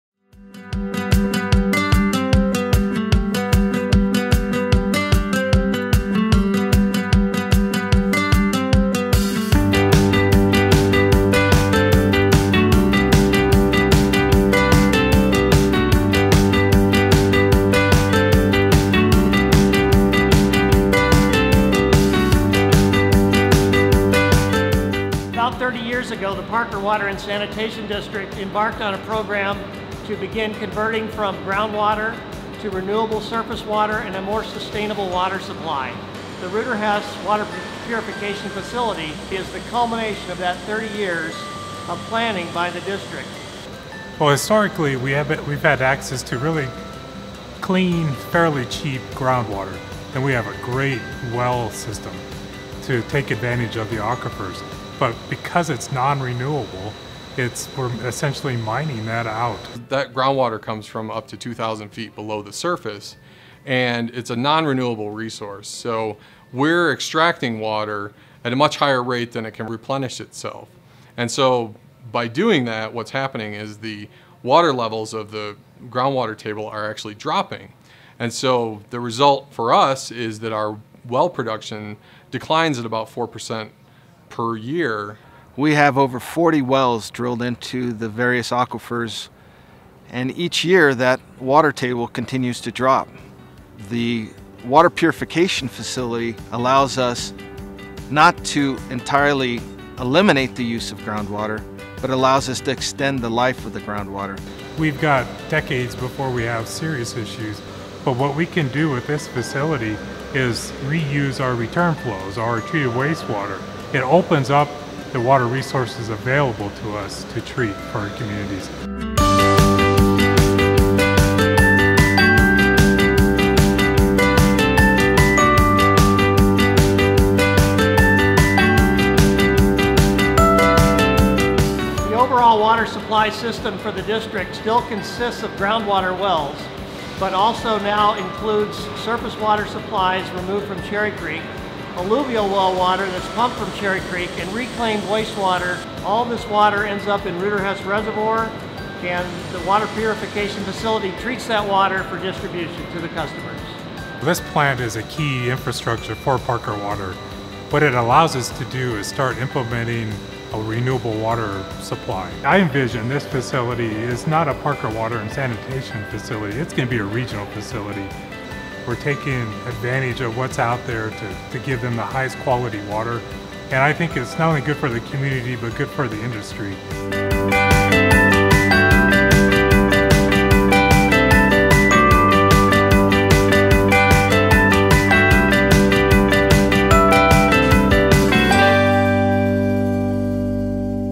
Rueter-Hess Water Purification Facility: Sustaining a Community’s Water Supply
Dewberry's Alan Pratt and Parker Water and Sanitation District's Ron Redd, Pieter Van Ry, and James Roche discuss how the Rueter-Hess Water Purification Facility benefits the community of Parker, Colorado.

Transcript: [Alan Pratt, associate vice president, Dewberry]
About 30 years ago, the Parker Water and Sanitation district embarked on a program to begin converting from ground water to renewable surface water and a more sustainable water supply. The Rueter-Hess Water Purification Facility is the culmination of that 30 years of planning by the district.

[Ron Redd, district manager, Parker Water & Sanitation District]
Historically, we've had access to really clean, fairly cheap ground water and we have a great well system to take advantage of, the aquifers. But because it's nonrenewable, it's essentially mining that out.

[Pieter Van Ry, director of engineering, Parker Water & Sanitation District]
That ground water comes from up to 2,000 feet below the surface and it's a nonrenewable resource. We're extracting water at a much higher rate than it can replenish itself. By doing that, what's happening is the water levels of the ground water table are actually dropping. The result for us is that our well production declines at about 4% per year.

[James Roche, director of operations, Parker Water & Sanitation District]
We have over 40 wells drilled into the various aquifers. Each year, that water table continues to drop. The water purification facility allows us not to entirely eliminate the use of ground water but allows us to extend the life of the ground water.

We've got decades before we have serious issues but what we can do at this facility is reuse our return flows, our water. It opens up the water resources available to us to treat for our communities.

[Alan Pratt, associate vice president, Dewberry]
The overall water supply system for the district still consist of ground water wells but also now includes surface water supplies removed  from Cherry Creek, alluvial well water that's come from Cherry Creek, and reclaimed waste water, all this water ends up in Reuters-Hess Reservoir and the water purification facility treats that water for distribution to the customers.

This plant is a key infrastructure for Parker Water. What it allows us to do is start implementing a renewable water supply. I envision this facility is not a Parker Water and Sanitation Facility, it's going to be a regional facility. We're taking advantage of what's out there to give them the highest quality of water. I think it's not only good for the community but good for the industry.